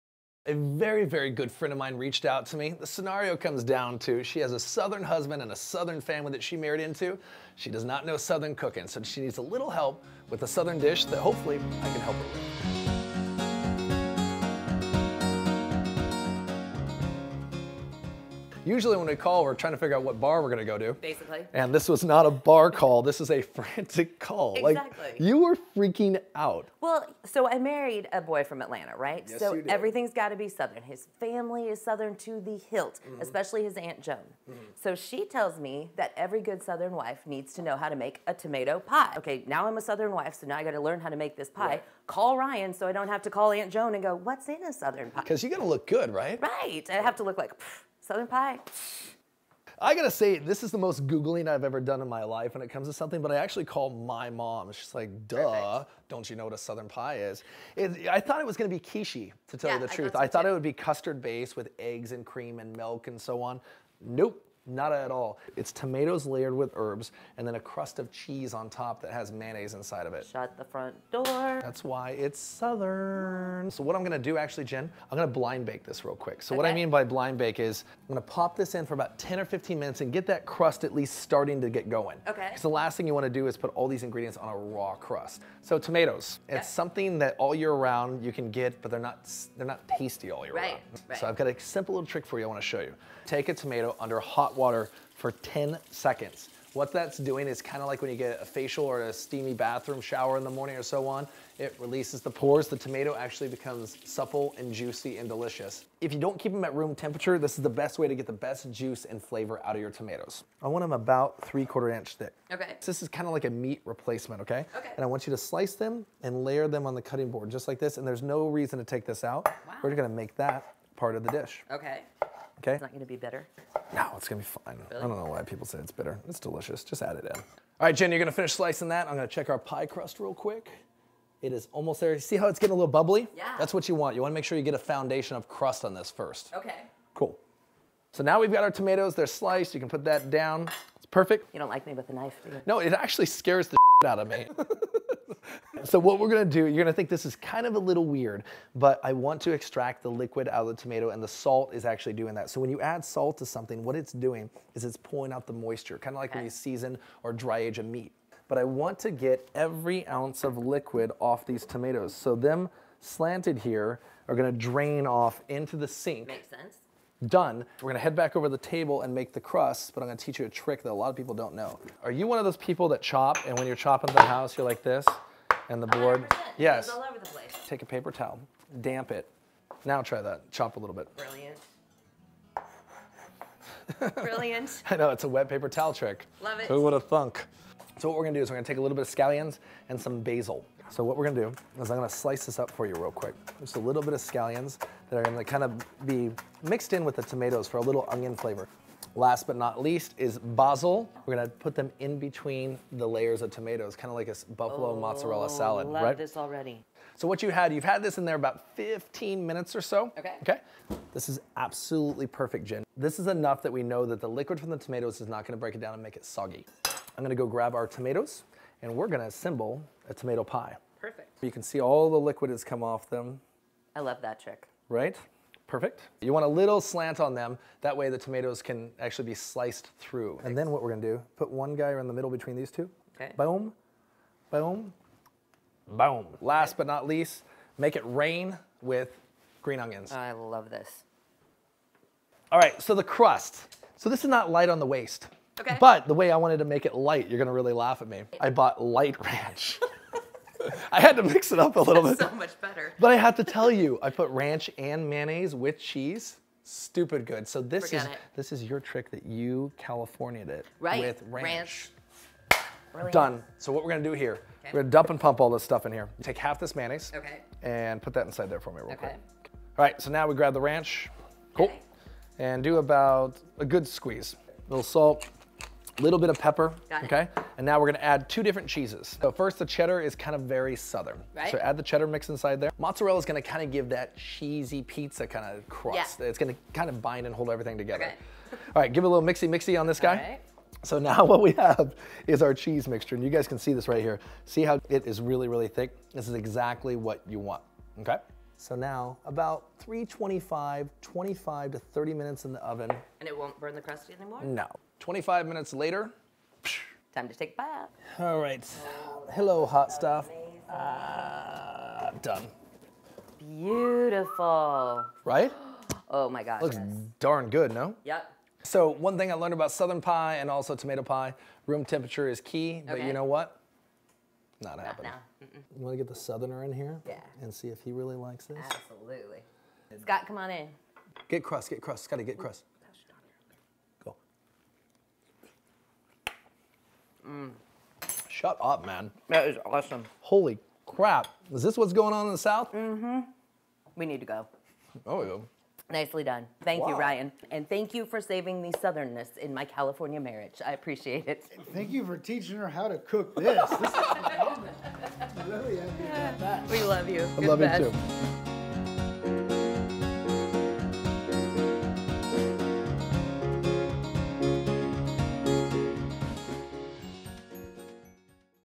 pic your dish: southern tomato pie
Jennifer of American Canyon wanted help ingratiating herself into her southern family with a classic dish.